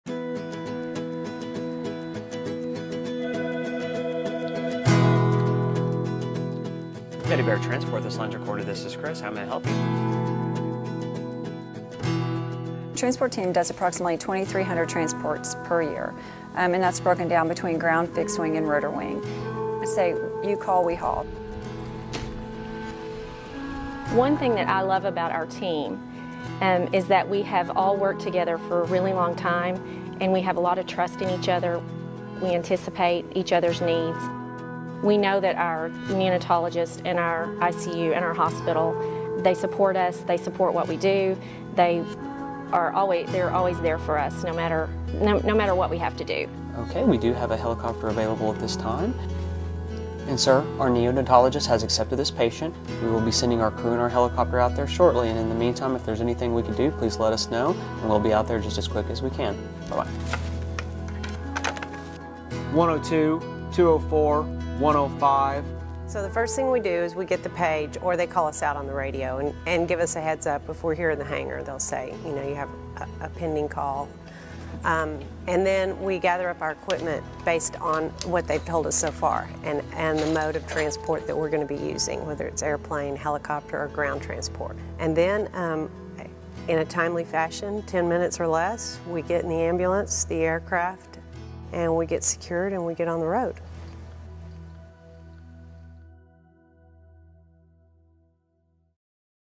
Teddy Bear Transport team
This is the Teddy Bear Transport team located at
Cook Children’s Medical Center